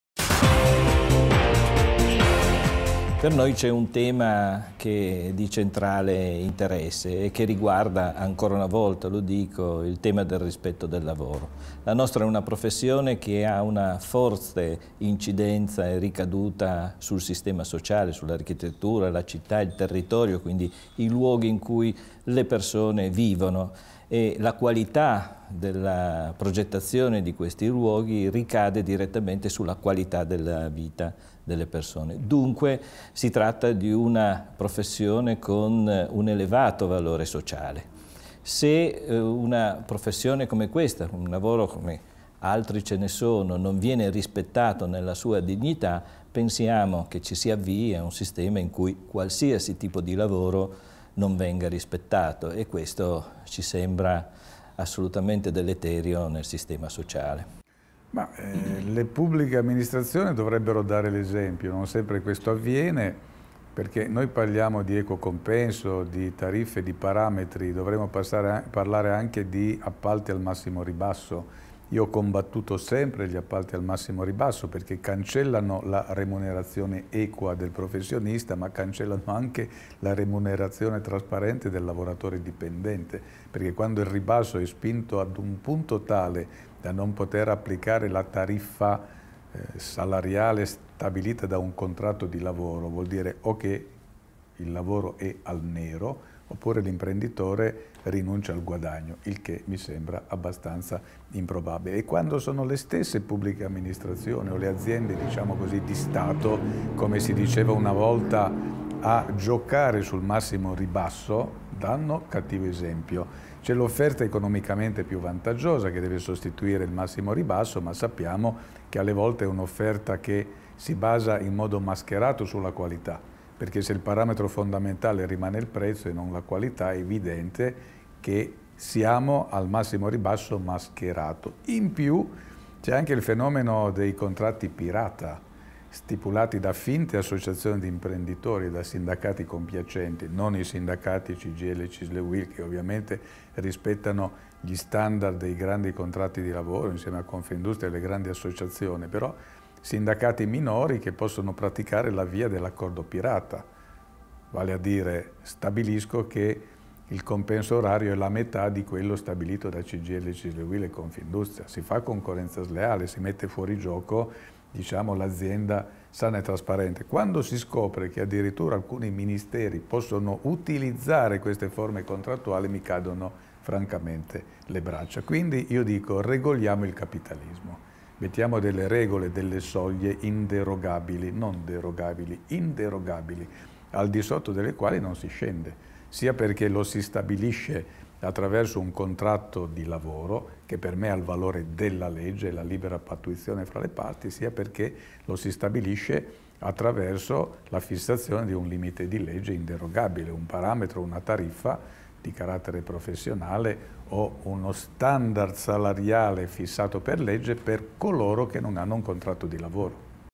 Cesare Damiano e il valore sociale dell'architetto
La qualità della progettazione dell'architettura, della città e del territorio incide profondamente sulla qualità della vita delle persone. Come fare perché il valore sociale dell'architetto venga pienamente riconosciuto? L'onorevole Cesare Damiano, presidente della Commissione Lavoro della Camera e ospite dell'incontro organizzato dalla Rete delle Fondazioni degli Ordini degli Architetti, risponde a Giorgio Giani, presidente della Fondazione per l'architettura.